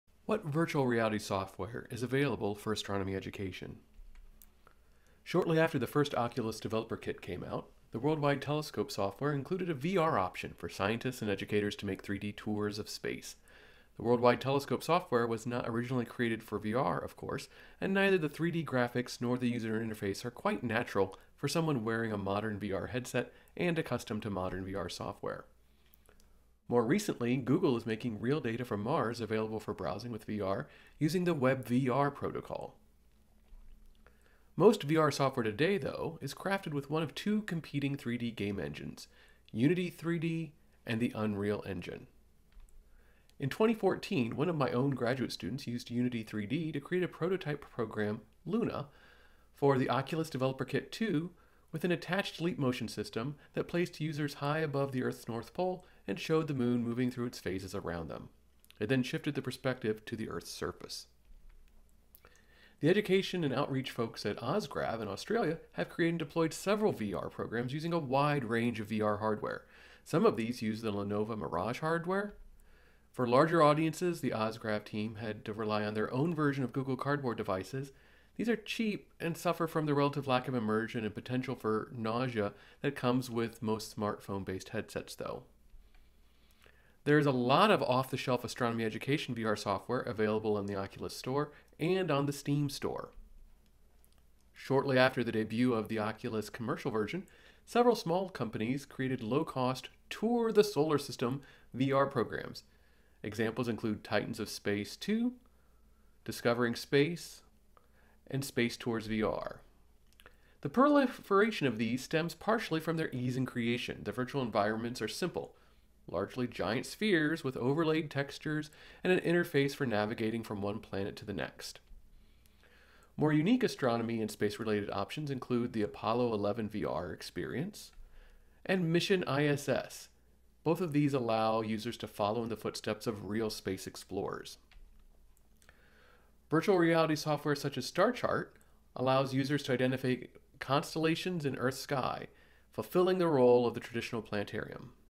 Part 3 - What virtual reality software is available for astronomy education?
This is Part 3 in a series of videos from a presentation I gave virtually at the 2020 American Astronomical Society meeting and adapted from a book chapter I wrote about education in the metaverse.

Crider, A. (2020). Astronomy Education in Virtual Worlds and Virtual Reality.  In Impey, C., & Wenger, M. (Eds.) Astronomy Education – Online Formal and Informal Learning (pp. 4-1 to 4-20). Institute of Physics: Bristol.